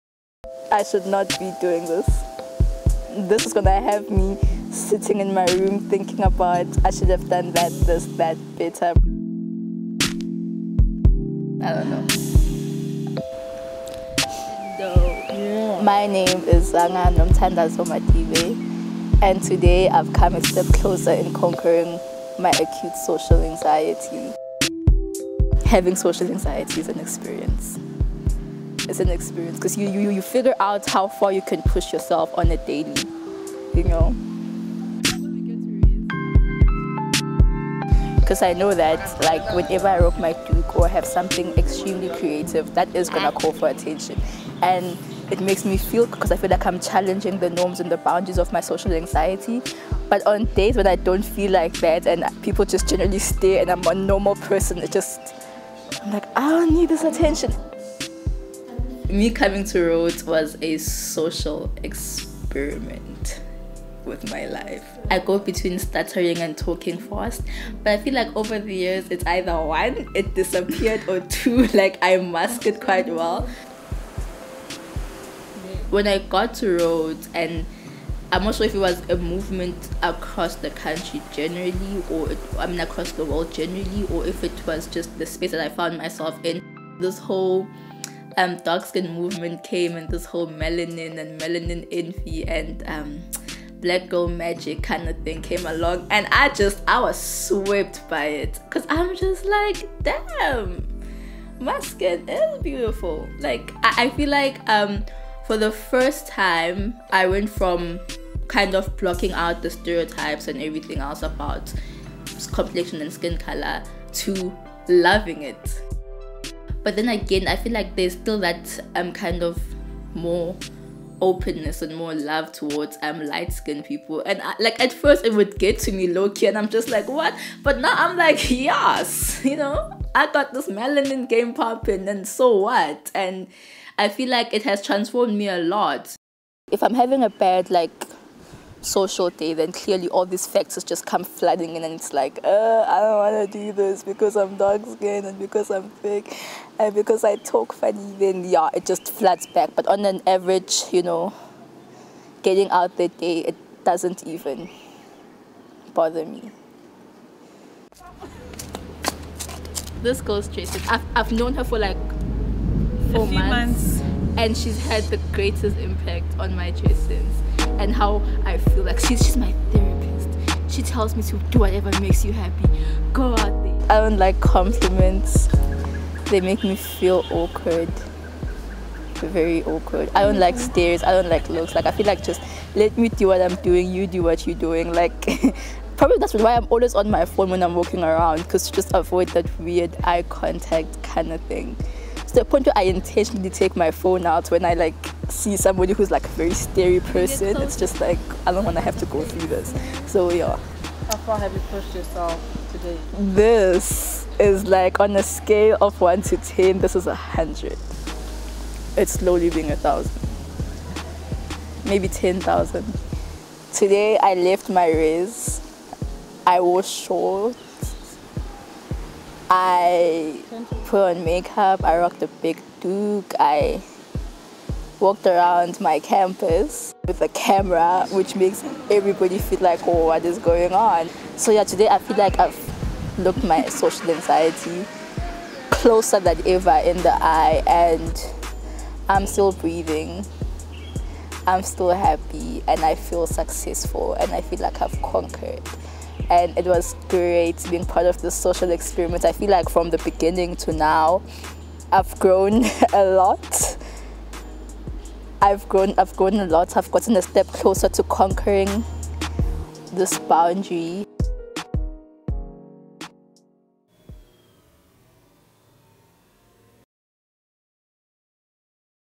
Socially Anxious Extrovert
Dzanga Nomthandazo Matibe is a 19-year-old student at the University Currently Known as Rhodes, a space to which she came as a social experiment on her life.

UCKAR has exposed her to a discourses that have enriched her self perception as an extremely melanated, full figured woman who stutters and talks very fast – parts of her that society tends to judge as bad.

As an extrovert, she is energised by the external world of people, but acute social anxiety – and sometimes the criticised parts of her – inhibit her enjoyment of social spaces. Add to that being followed with around campus with a big camera.

Special thanks:
Mutale Mpuku